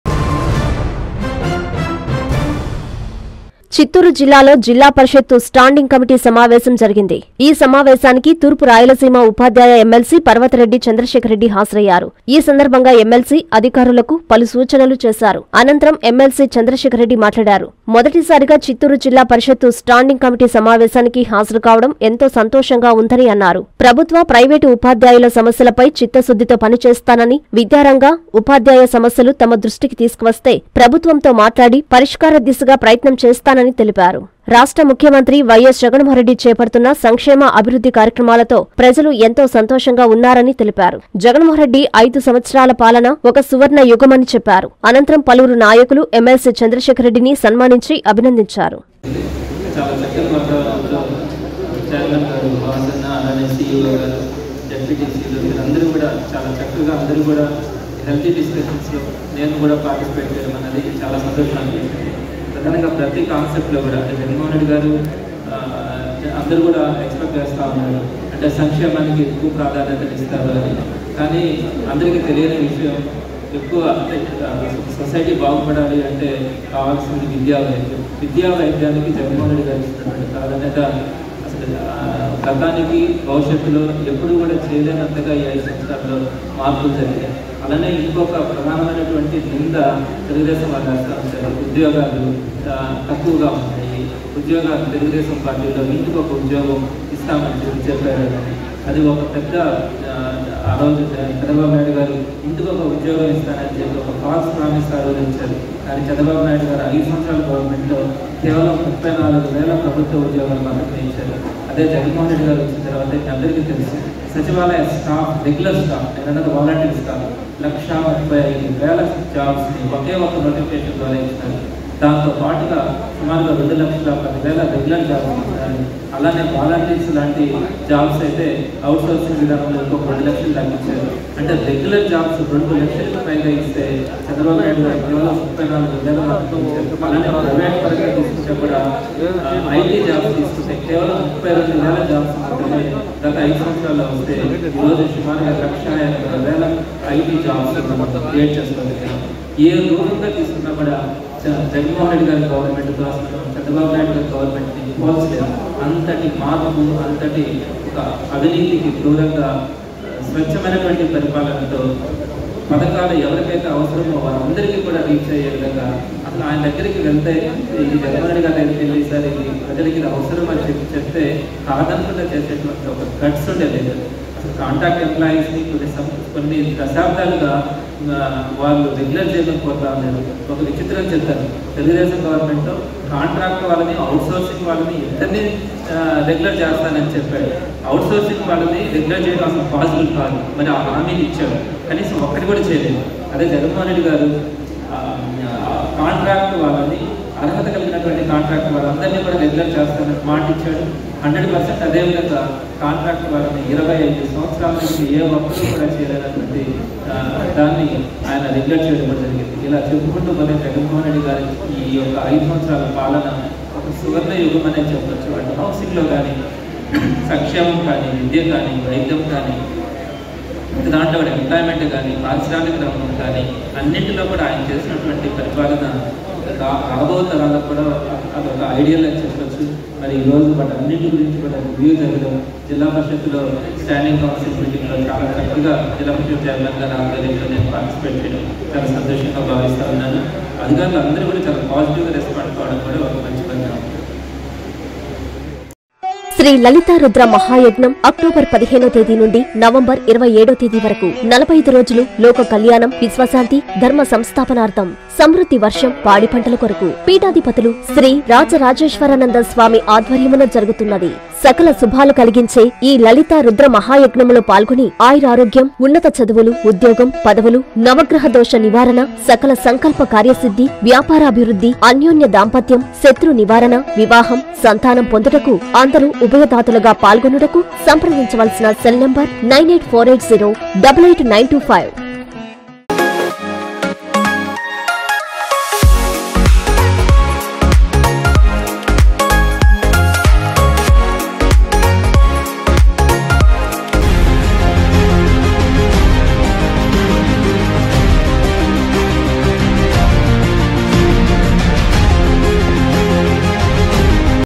సీఎం పాల‌న‌...ఒక సువ‌ర్ణ యుగం@N3NEWS
- అభివృద్ధి, సంక్షేమం ఆయ‌న‌కే సాధ్యం
- ప్రభుత్వ ప్రైవేటు ఉపాధ్యాయుల స‌మ‌స్య‌ల్ని ప్ర‌భుత్వం దృష్టికి తీసుకెళుతా
- ఎమ్మెల్సీ ప‌ర్వ‌త‌రెడ్డి చంద్ర‌శేఖ‌ర్‌రెడ్డి
- చిత్తూరు జిల్లా పరిషత్ స్టాండింగ్ కమిటీ సమావేశంలో అధికారులకు పలు సూచనలు చేసిన ఎమ్మెల్సీ